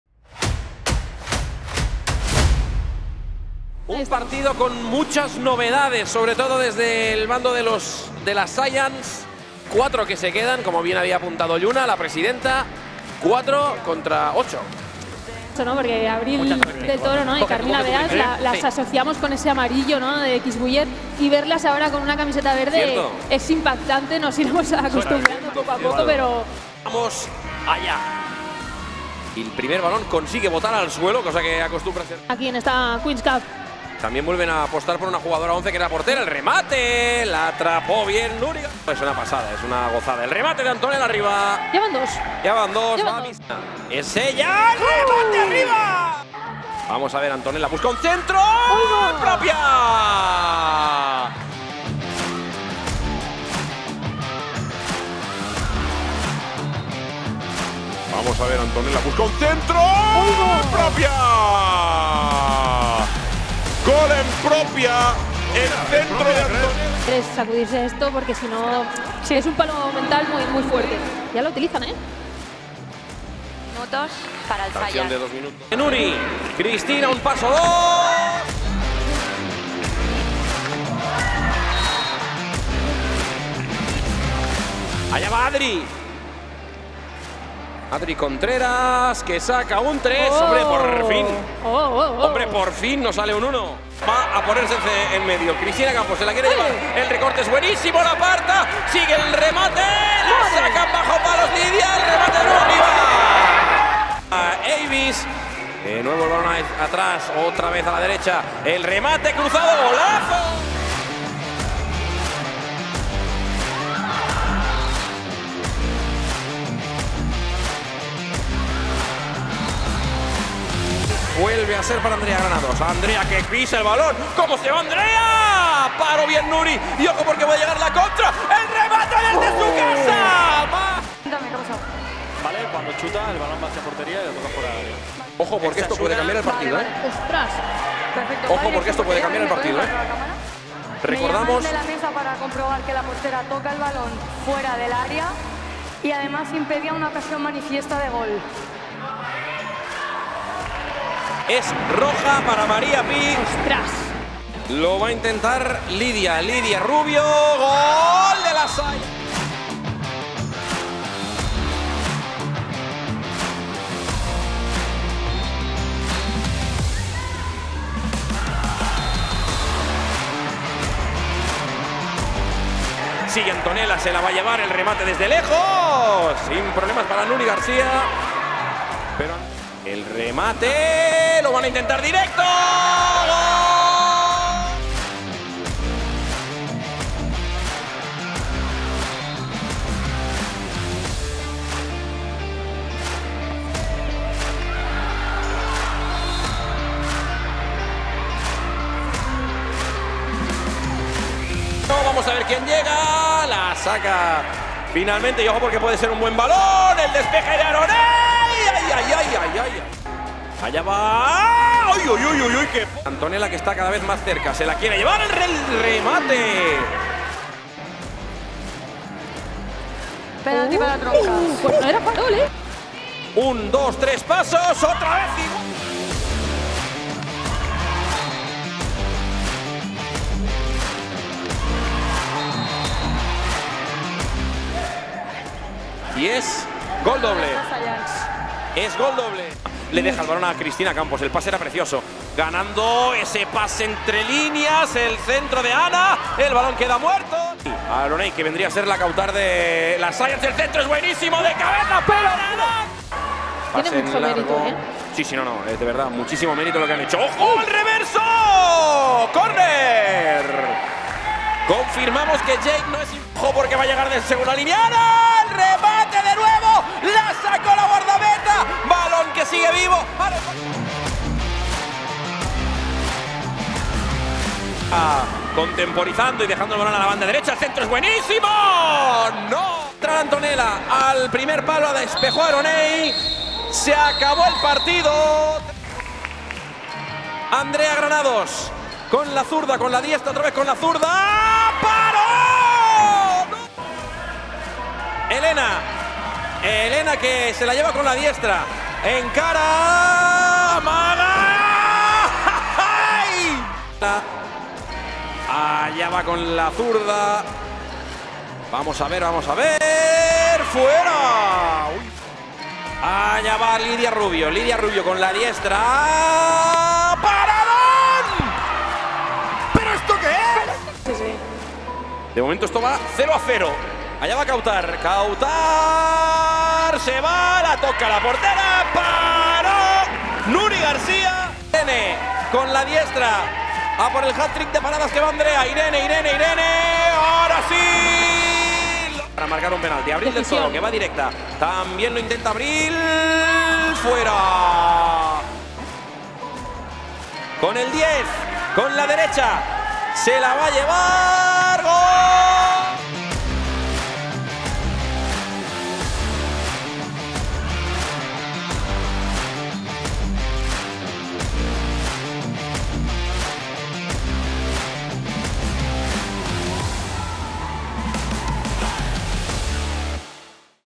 Saiyans FC de LLUNA CLARK VS Las Troncas FC de VIOLETA | Resumen Partido F. Grupos J1 (3-3) (P. 2-0)
Mejores momentos del cuarto partido de Fase de Grupos J1: Saiyans FC de LLUNA CLARK VS Las Troncas FC de VIOLETA | 3-3 (Penaltis 2-0).